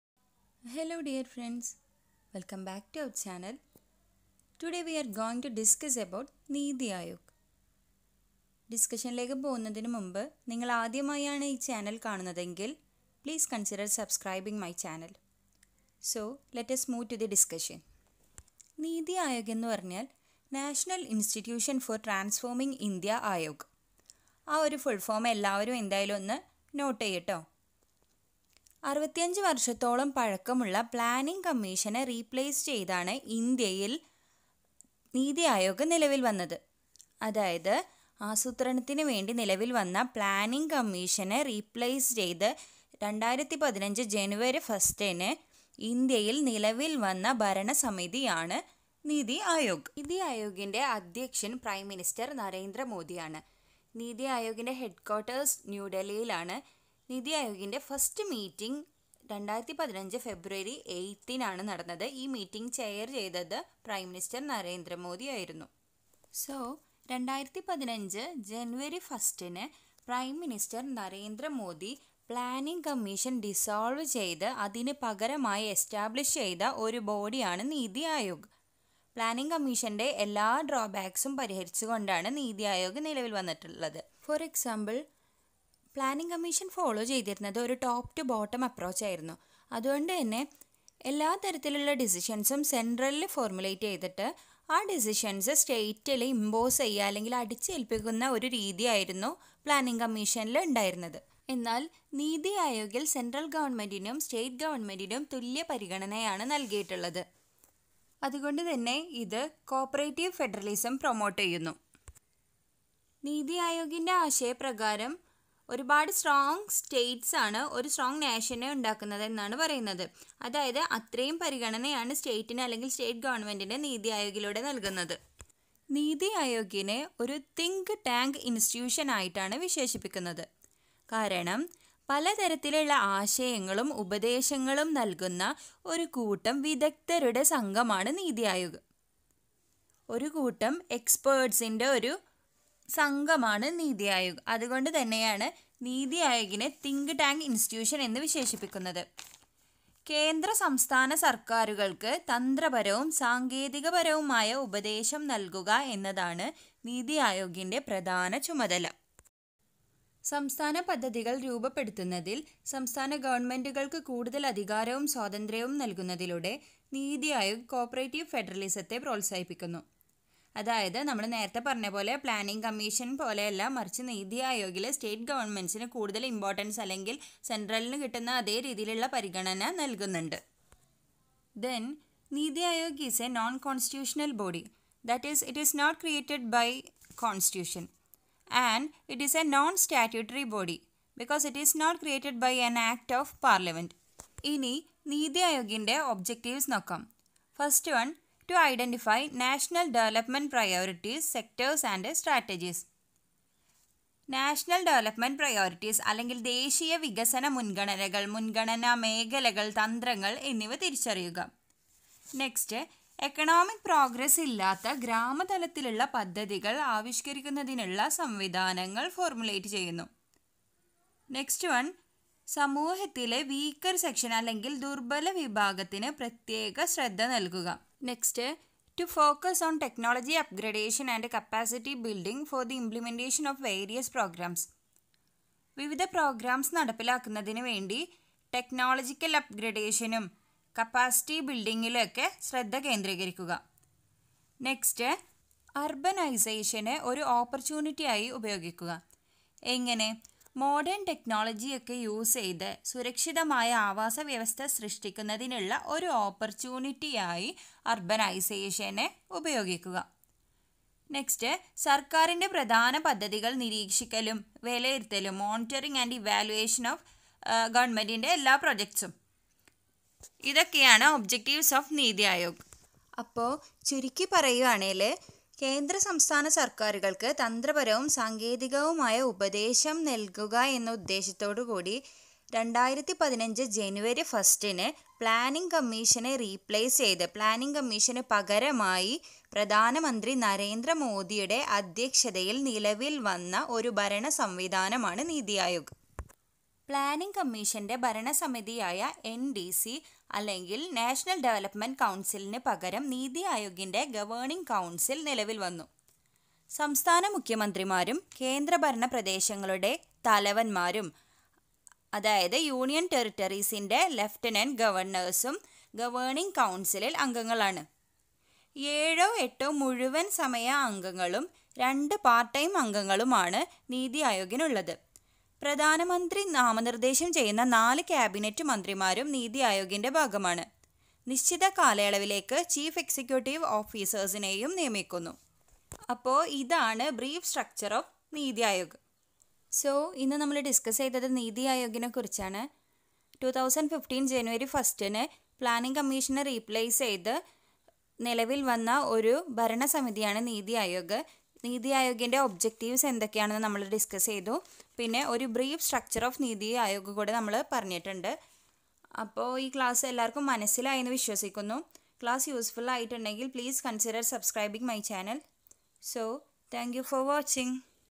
Niti Aayog / M. com Business environment /Malayalam
Clear explanation of Niti Aayog in Malayalam

niti aayog
planning commission
business environment